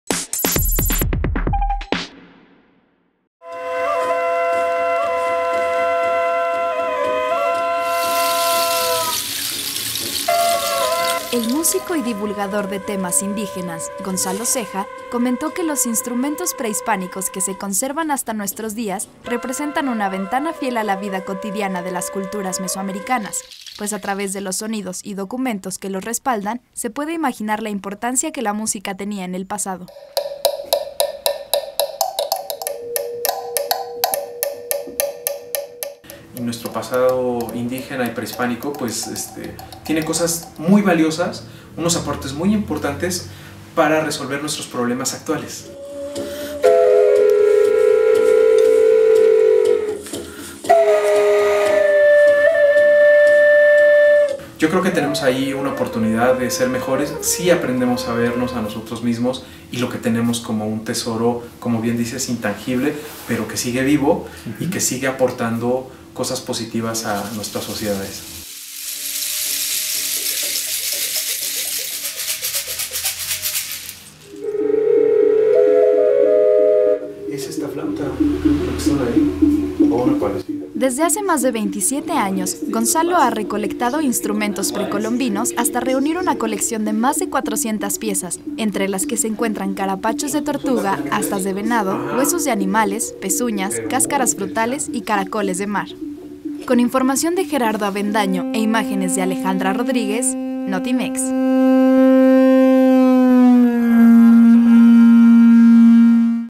Instrumentos prehispánicos, una ventana al pasado
Los instrumentos prehispánicos que se conservan hasta nuestros días representan una ventana fiel a la vida cotidiana de las culturas mesoamericanas